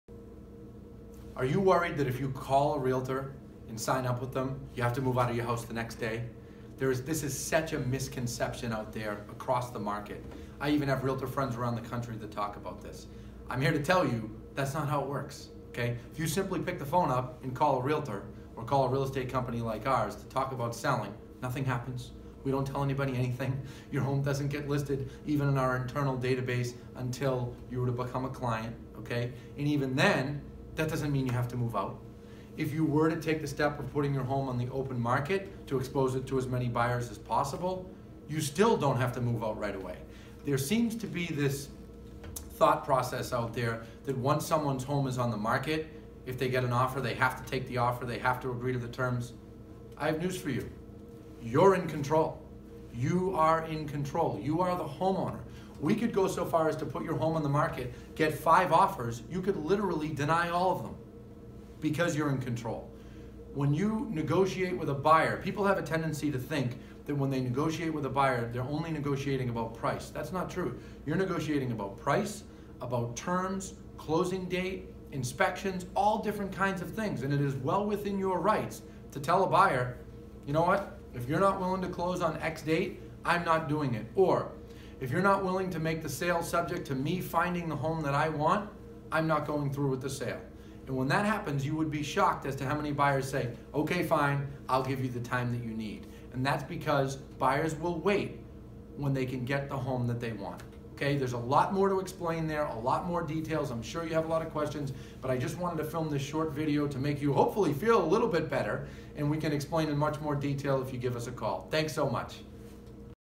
Listing does not mean Move Out
Just because you have called a realtor to get more information about selling does not mean you have to move out right away!